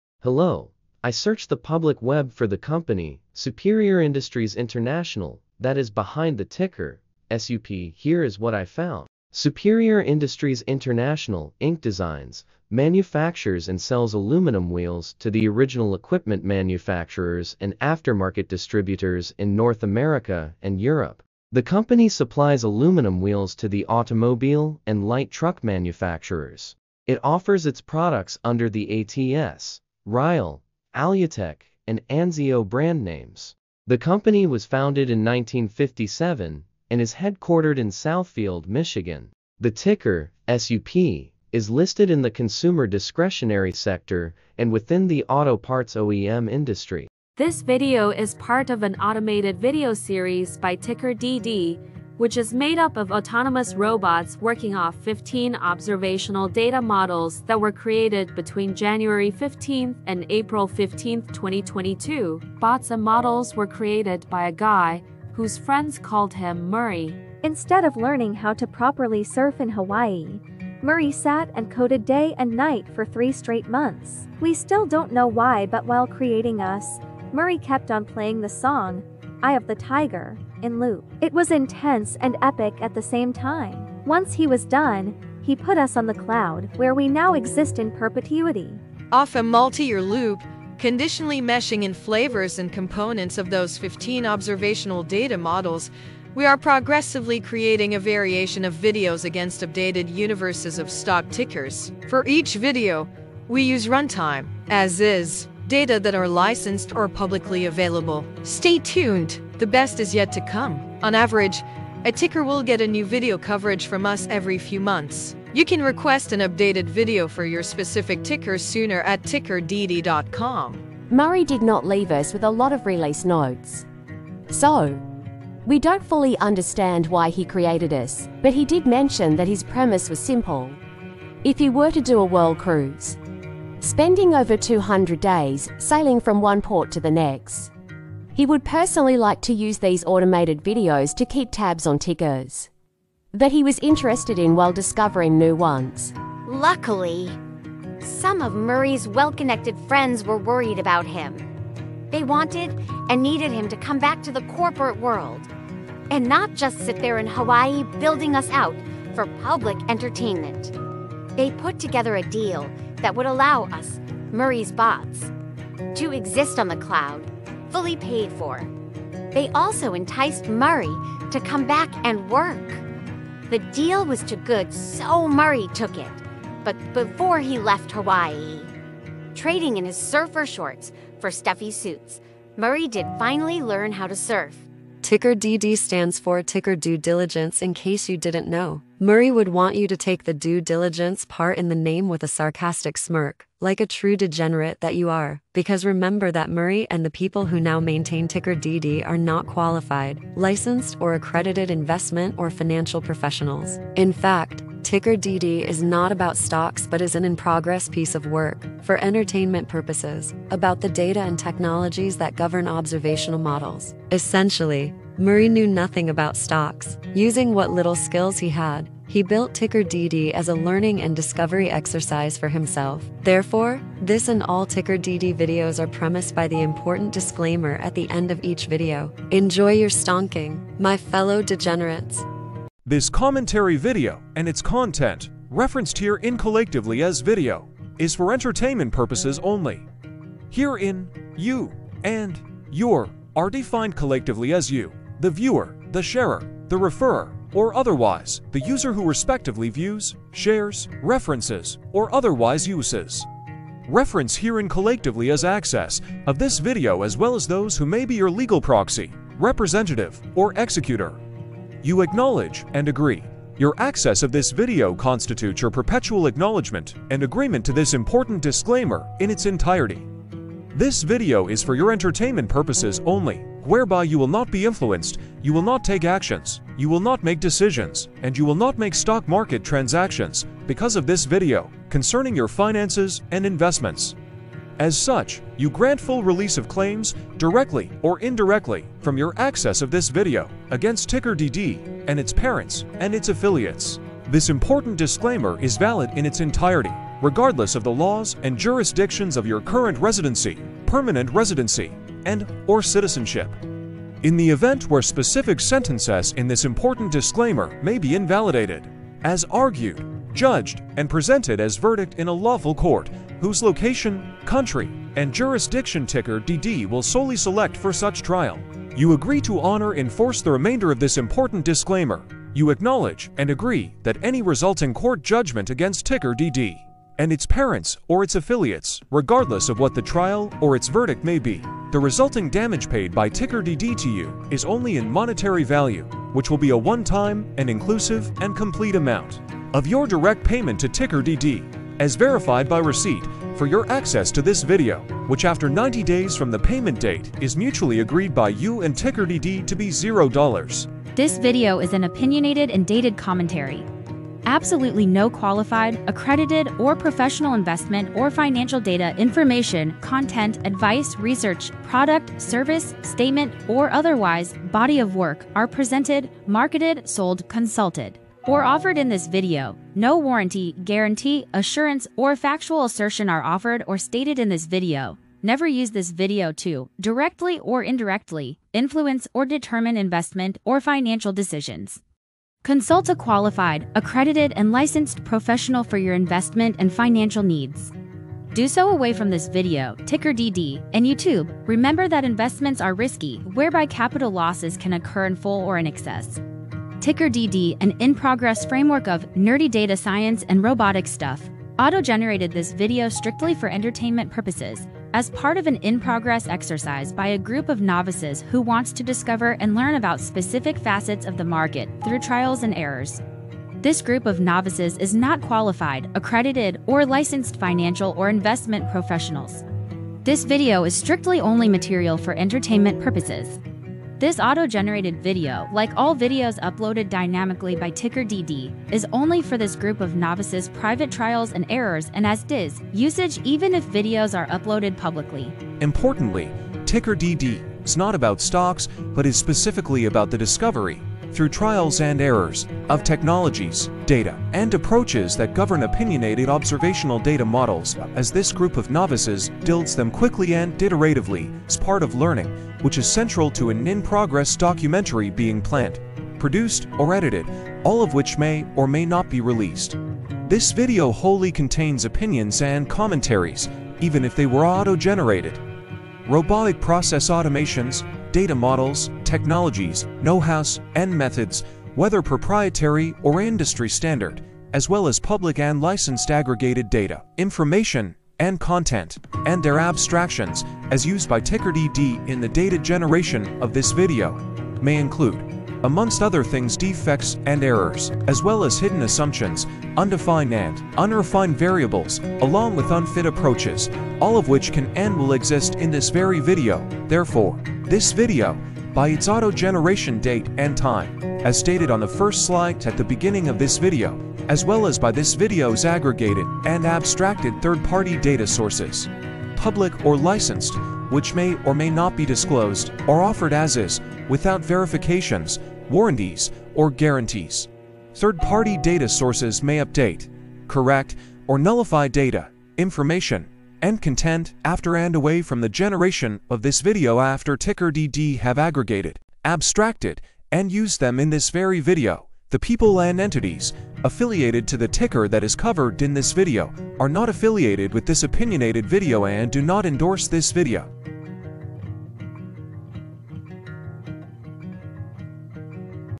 What is the Company behind the SUP Stock Ticker? about Superior Industries International
What is the Company behind the SUP Stock Ticker? About Superior Industries International. Entity Profile for Superior Industries International